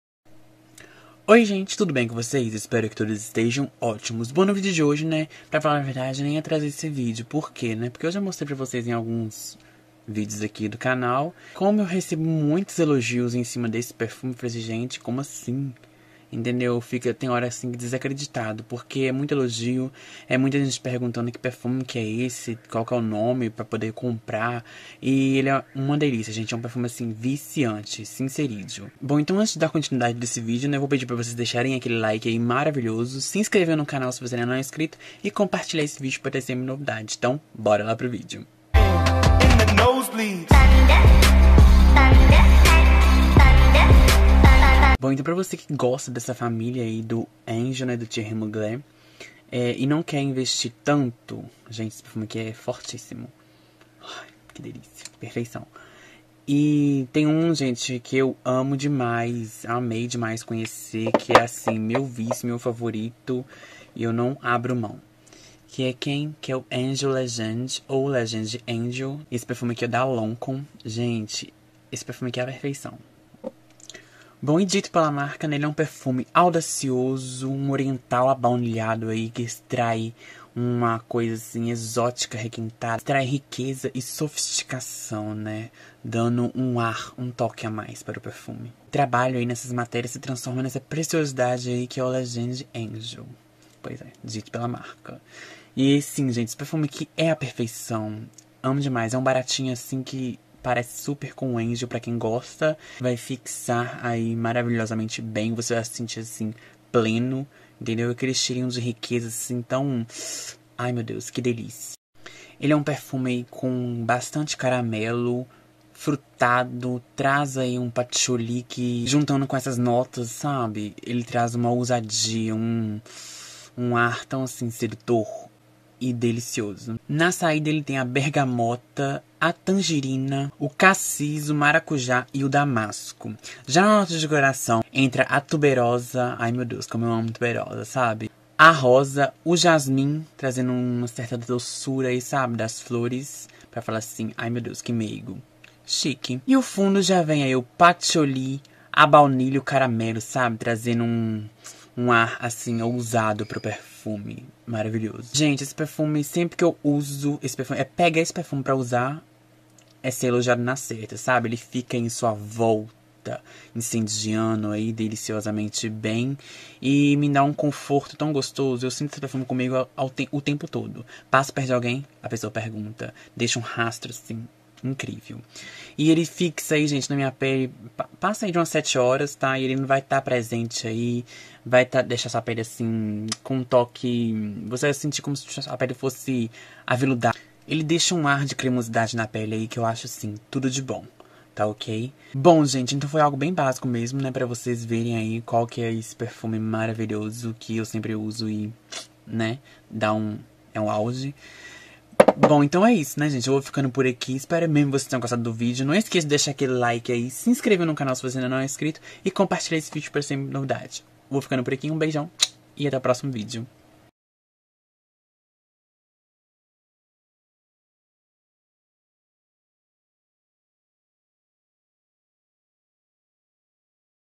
Quando uso Todos Perguntam Perfume Parecido com Angel
Perfume super parecido com o Angel mugler que todos querem saber quando uso ! Fixa muito e deixa um rastro maravilhoso !

Legend Angel Lonkoom